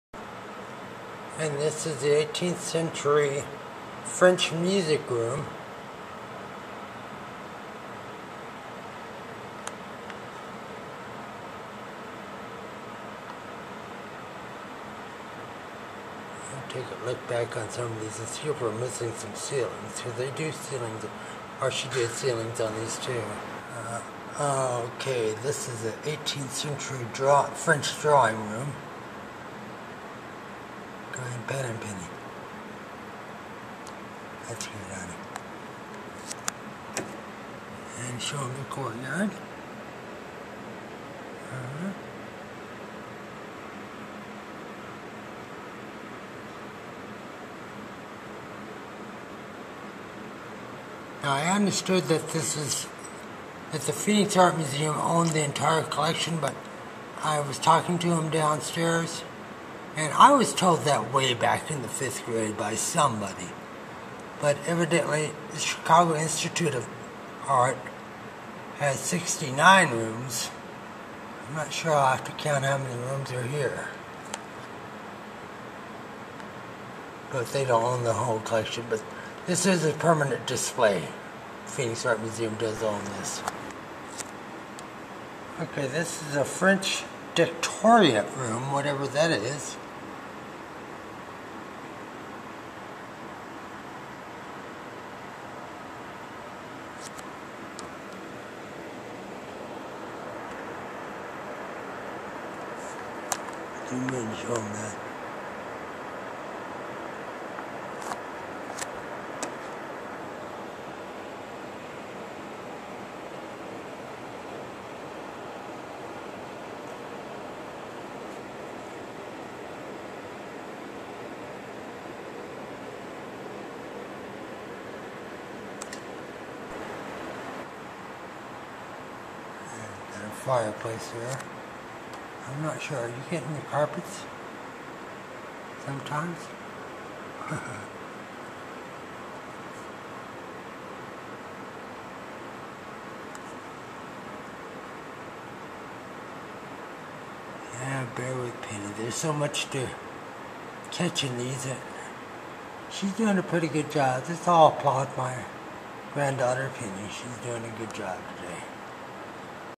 ❤இڿڰۣ-❤Thorne Miniature Rooms❤இڿڰۣ-❤
☼PLEASE READ☼

Hi Ladies & Gentlemen it's GranDaughter Penny♥ & I have an Extra Special Collection of Sweet Treats for you♥♥
Hold on we're getting
Next we have
The Thorne mini rooms #4
The Phx Art Museum Collection

Legacy

Due to poor health, Thorne closed her studio in March 1966, donating her remaining works to charity. She died in Chicago in June of that year, and was buried in Rosehill Cemetery.

Most of Thorne's works were donated to museums and remain there, although some were auctioned off in 1985. Thorne herself arranged for thirty of the rooms to be auctioned off for charity in 1963.

Approximately one hundred Thorne rooms are known to exist. The Art Institute of Chicago holds 68 Thorne rooms, which originally occupied a dedicated wing but are now housed in a large room in the building's lower level.An additional 20 are held by the Phoenix Art Museum, and nine by the Knoxville Museum of Art. The remaining two are at the Indianapolis Children's Museum, and the Kaye Miniature Museum in Los Angeles. In addition to these, a bar that Thorne auctioned off for charity in the 1950s is at the Museum of Miniature Houses in Carmel, Indiana.